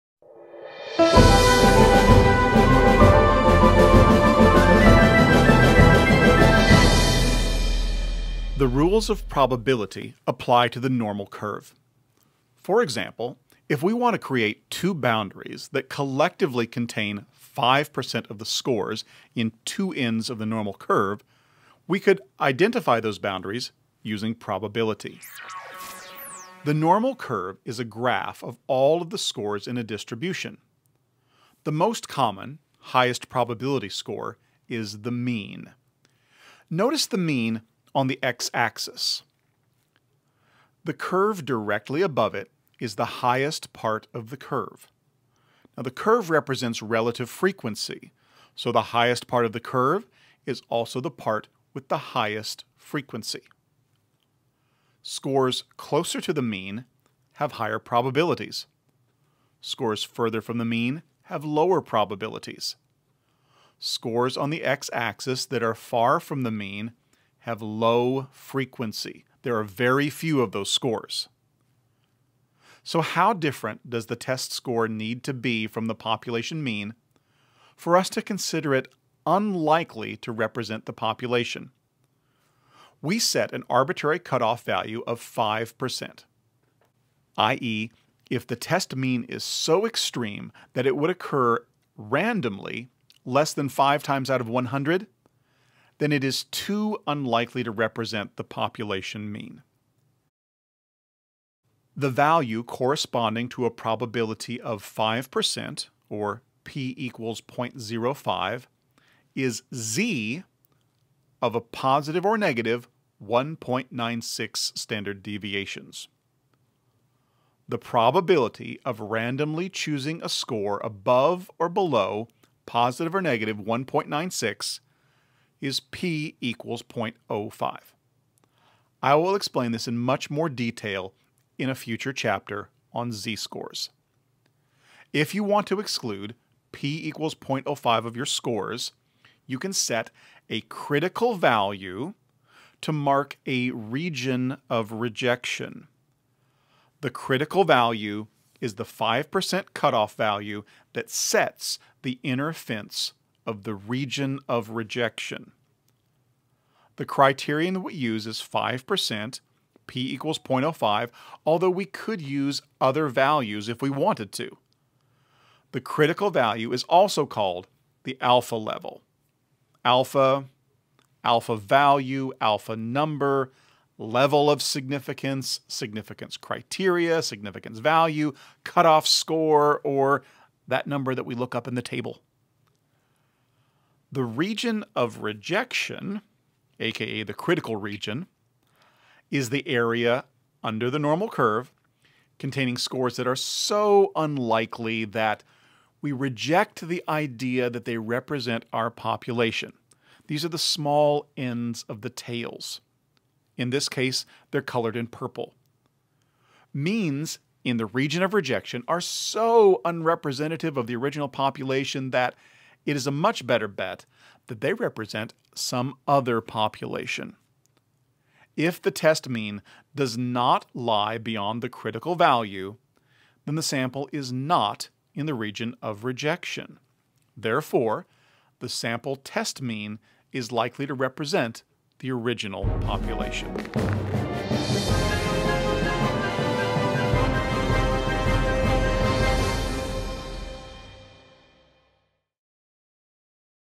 Probability and the Normal Curve – How to Identify a Critical Value (7-9)
The rules of probability apply to the normal curve. The normal curve is a graph of all the scores in a distribution. The most common (highest probability) score is the mean. The curve represents relative frequency, so the highest part of the curve is also the part with the highest frequency. Scores closer to the mean have higher probabilities. Scores father from the mean have lower probabilities. Scores on the X-axis that are far from the mean have low frequency.

This video teaches the following concepts and techniques:
How to use the normal curve to identify a critical value

Link to a Google Drive folder with all of the files that I use in the videos including the Bear Handout and datasets. As I add new files, they will appear here, as well.